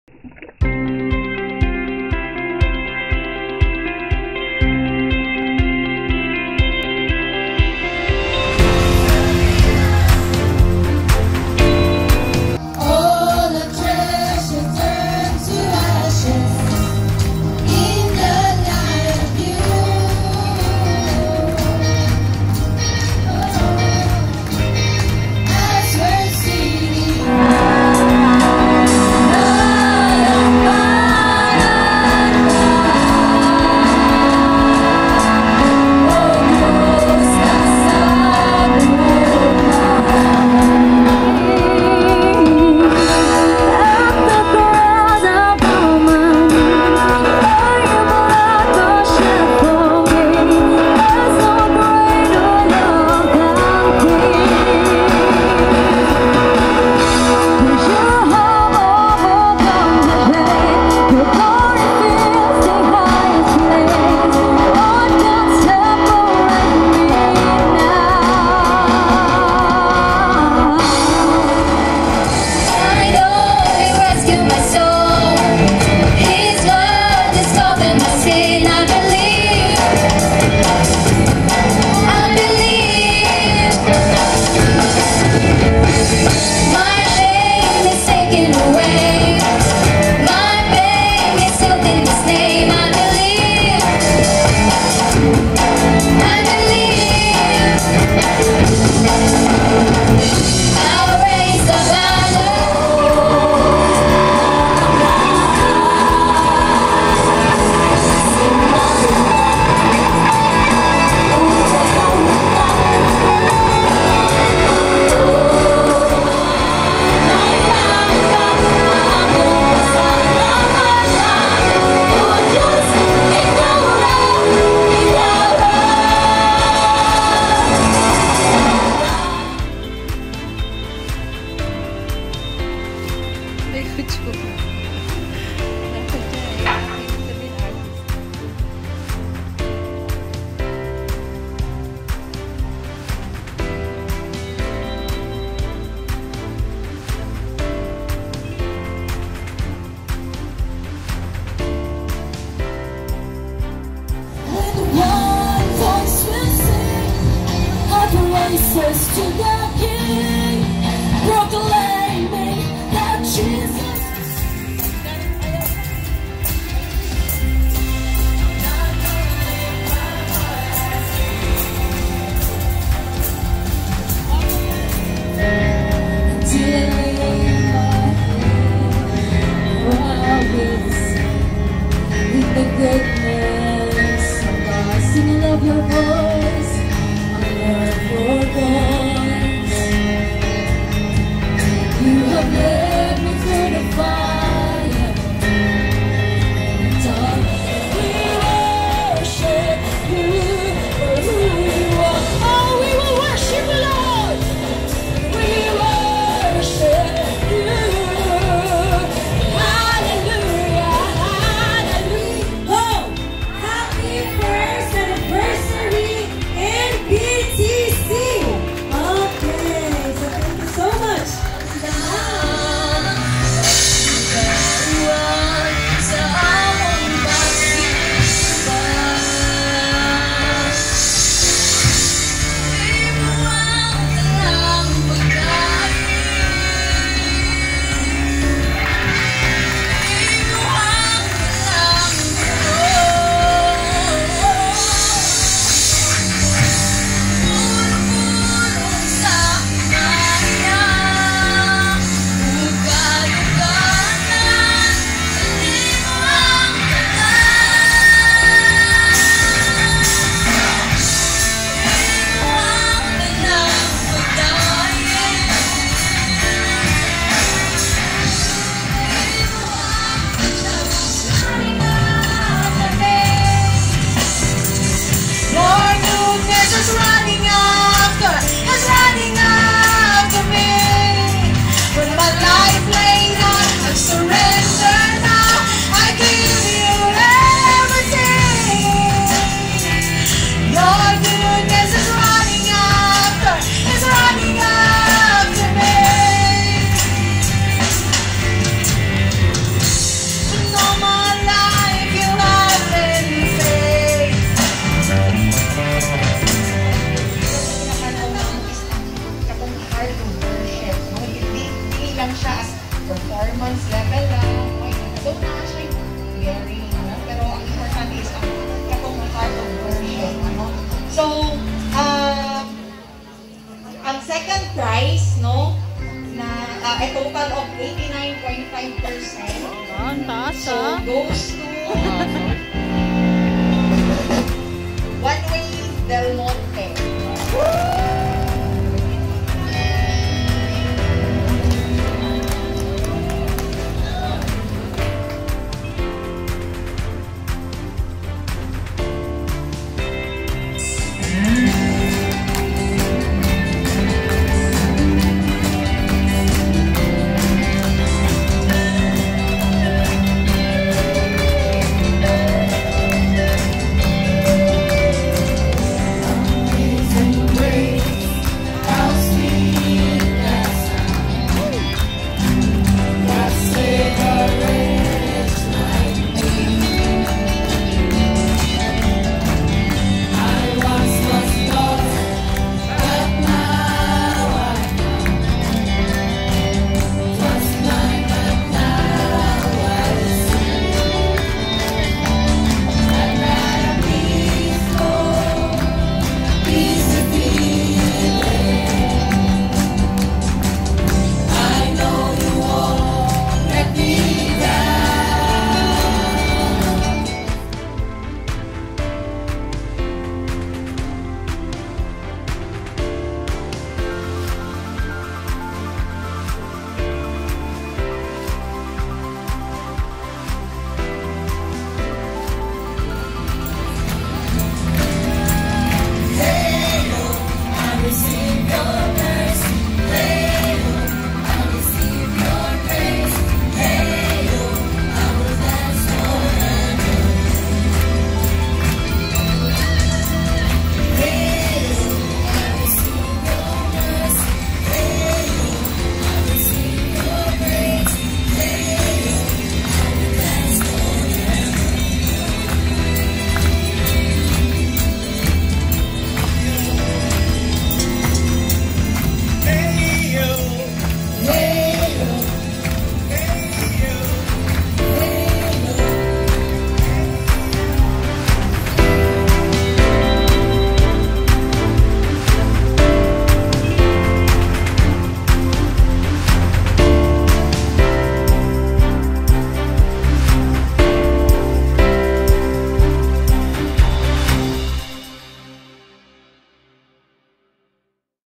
Samal Newman Bible Academy, 2021 First Anniversary HIGHLIGHTS
Samal Newman Bible Academy celebrated its first anniversary last March 20, 2021. Several worship teams from different churches performed during the Worship Fest.